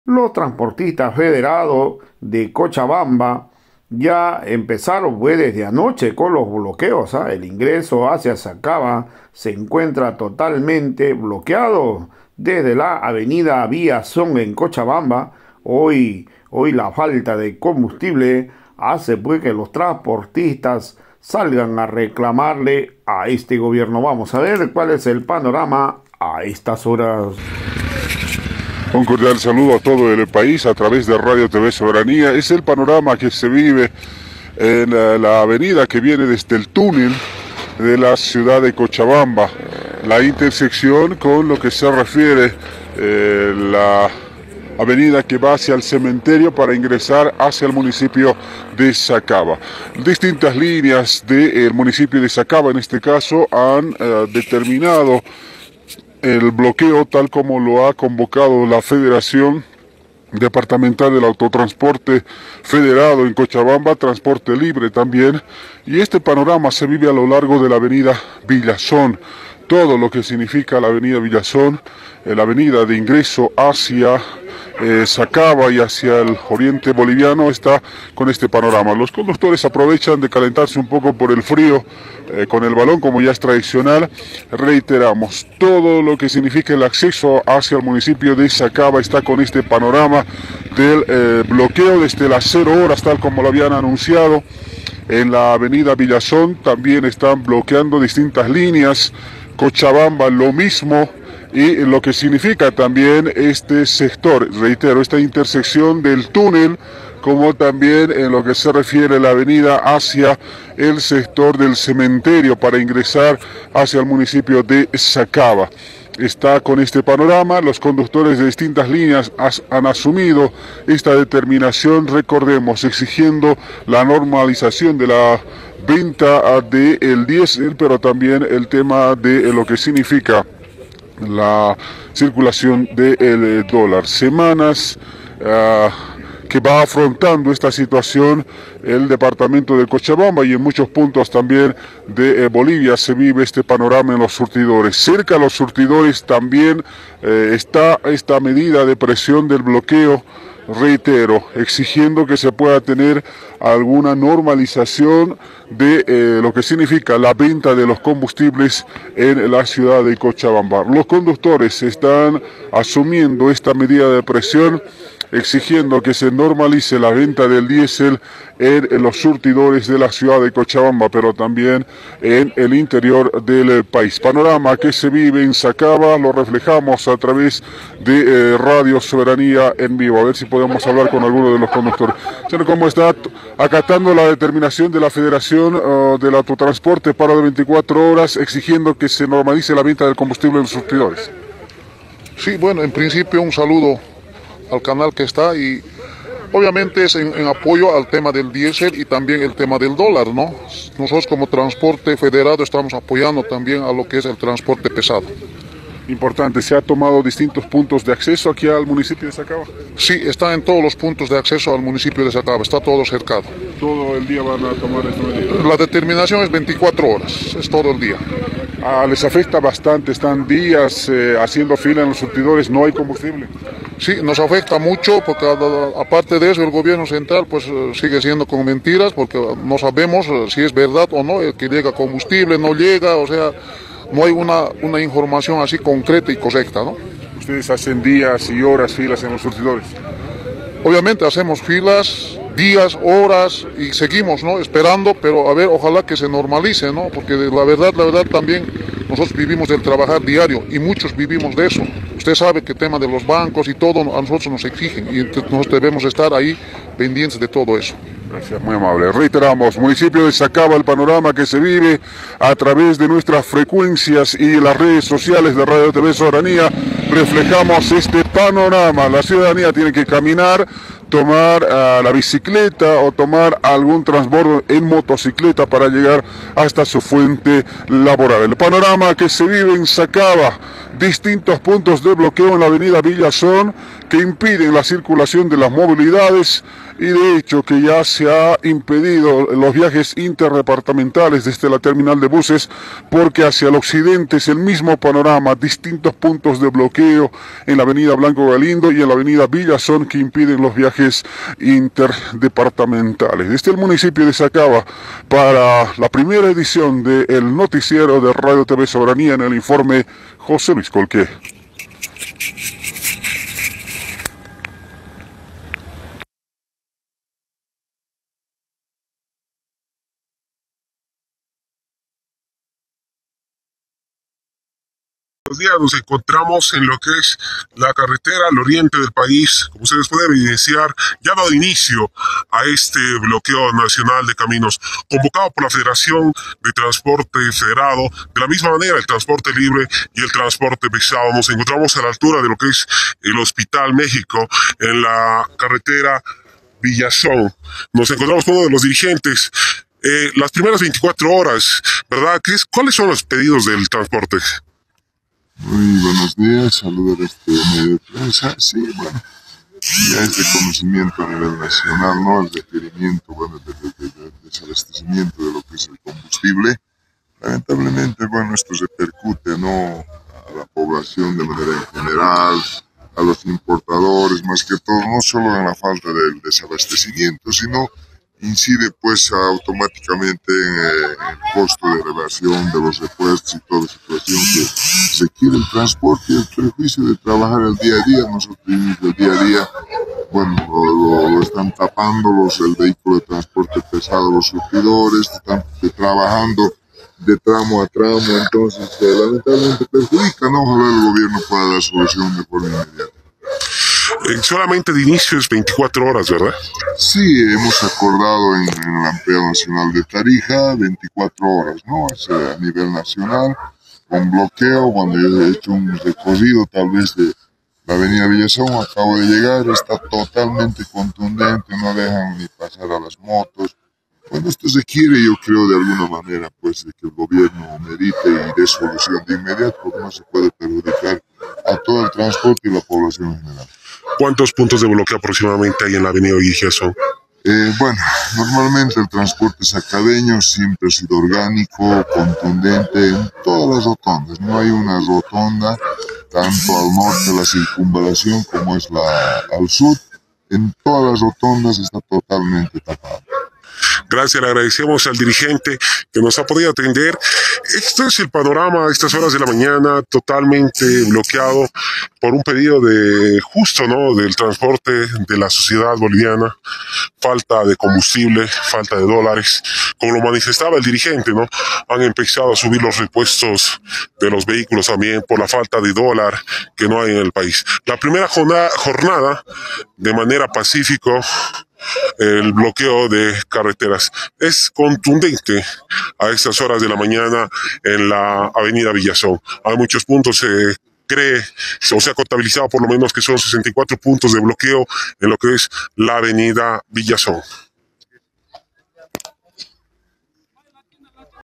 TRANSPORTISTAS FDERADO BLOQUEAN EL INGRESO DE SACABA AV. SECTOR VILLAZON EN COCHABAMBA..
PANORRAMA DEL BLOQU3O POR LA FALTA DE COMBUSTIBLES POR RESPONSABILIDAD DEL GOBIERNO DE ARCE..